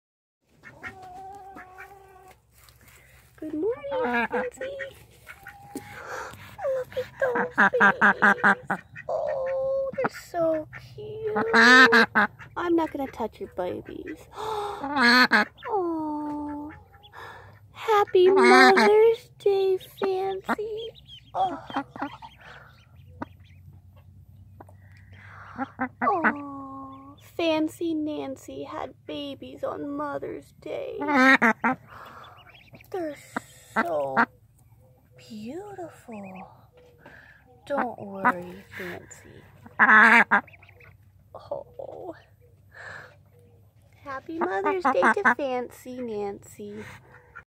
Fancy hatches ducklings on mother's day
We have been waiting and waiting and the day has finally arrived.Our Indian runner duck' s eggs hatch on the morning of mother's day. She has 10 babies! She's a first time mom.